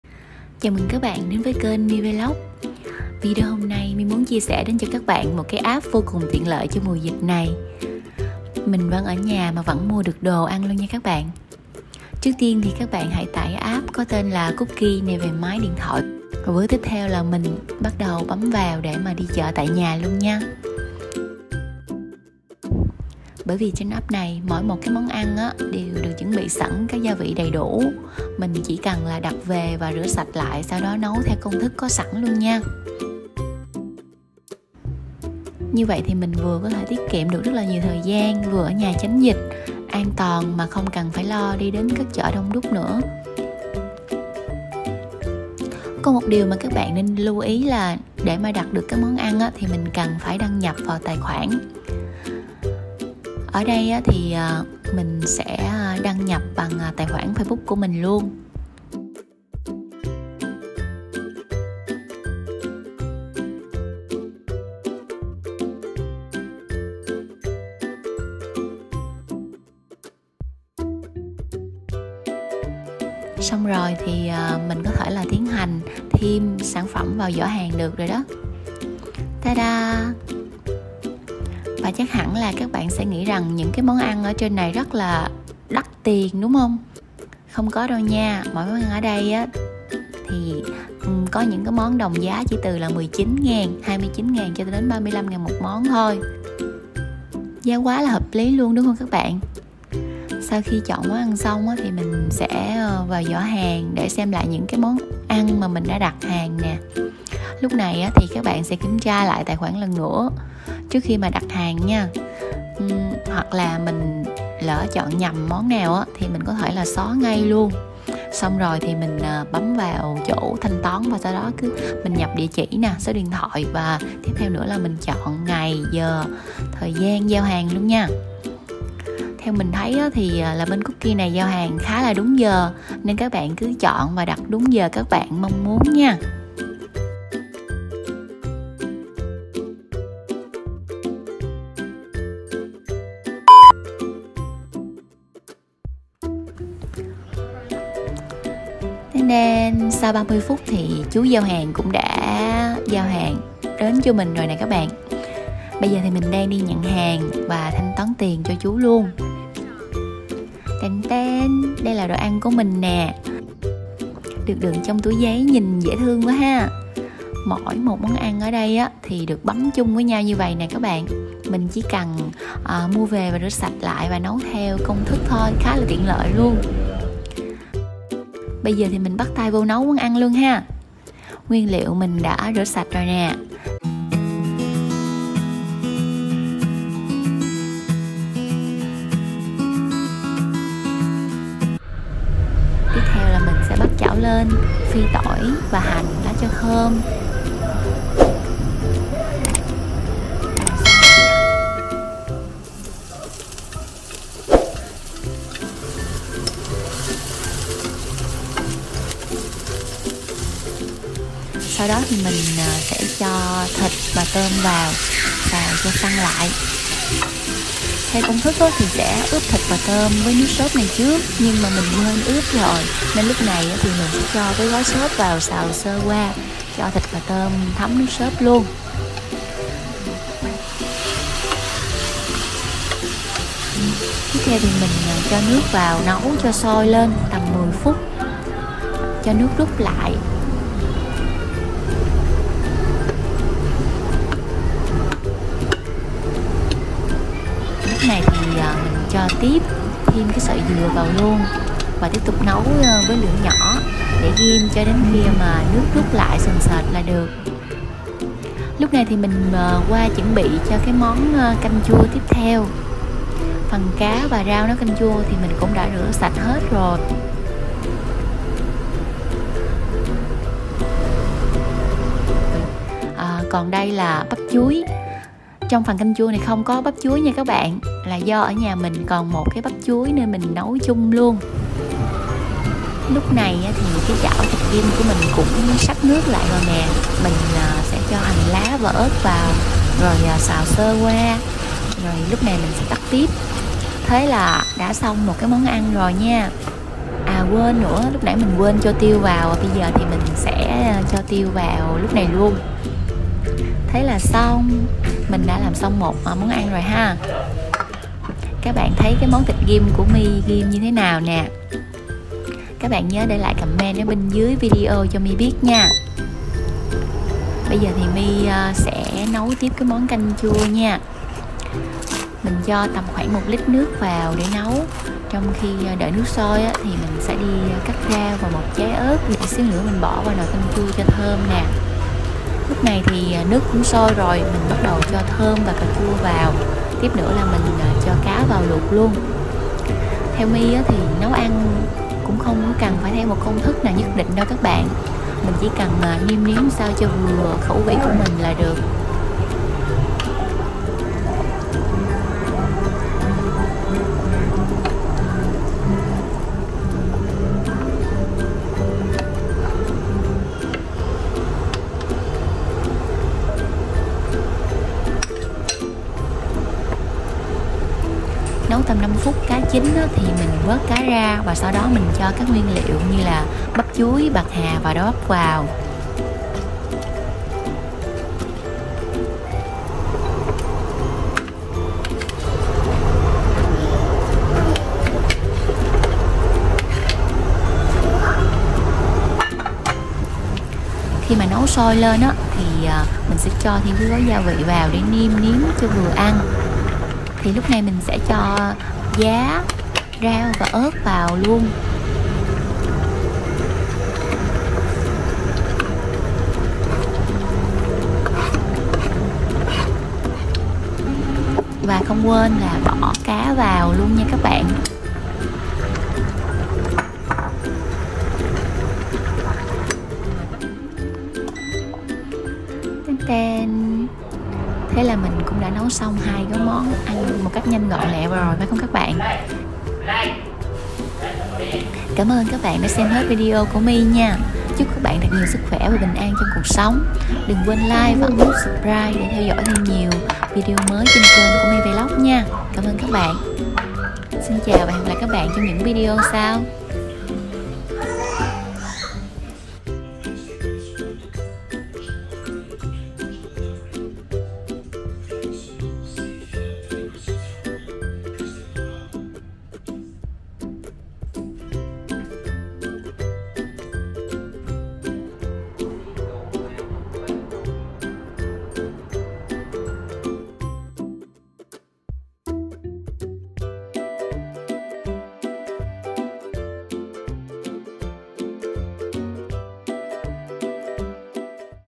Bạn nên biết sớm Cách Người Dân Sài Gòn Đi Chợ Mùa Dịch Covid-19 An Toàn | My Vlogs
Bản quyền thuộc về @My Vlogs
Nghiêm cấm mọi hình thức Reup.
Copyright @MyVlogs
Please Don't Re-Up.

Cảm ơn bạn đã xem video !